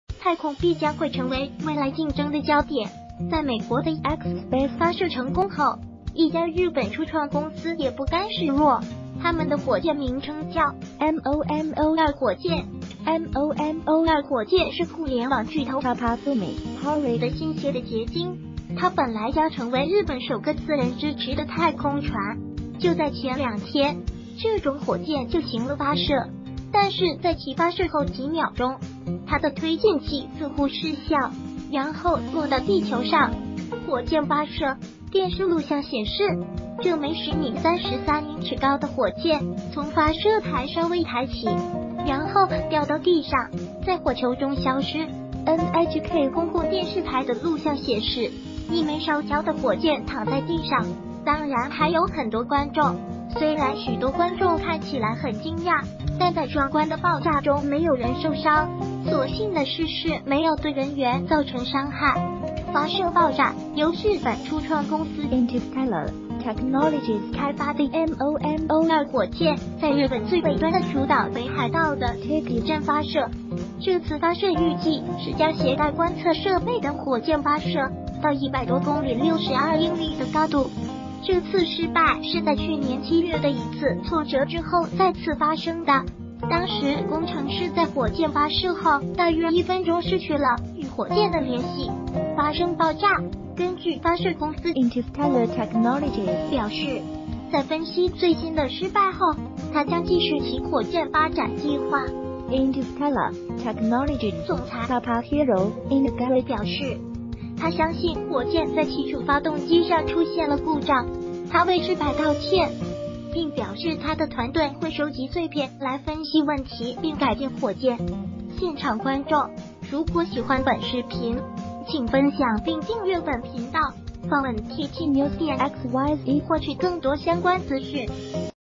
高達10米的火箭，發射後幾秒就在發射臺爆炸，現場很多觀眾
高達10米的火箭，發射後幾秒就在發射臺爆炸，現場很多觀眾高達10米的火箭，發射後幾秒就在發射臺爆炸，現場很多觀眾火箭 日本 地球 工程師 北海道